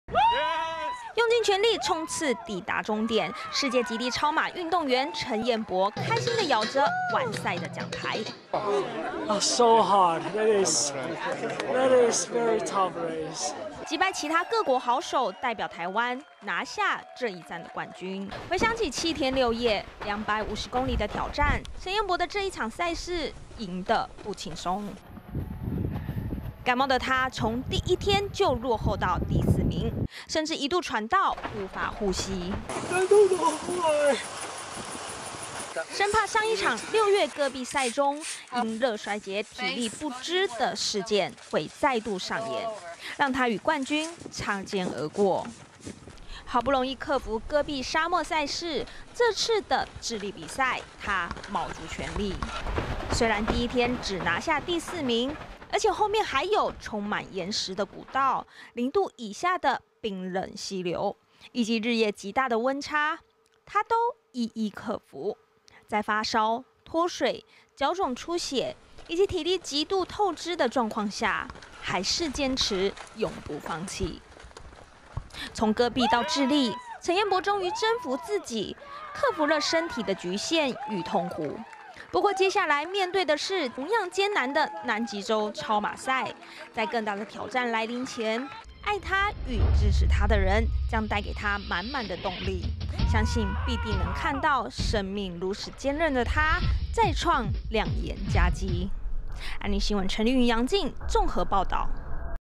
超馬好手陳彥博 南美洲智利勇奪冠軍
今年挑戰的四大極地，第三戰在南美洲智利的，
250公里高原馬拉松賽，經過7天6夜的奮戰之後，
最後終於奪得冠軍。也讓陳彥博目前在總積分，
暫時領先，接下來就看南極最終站，
能不能再度突破自我極限。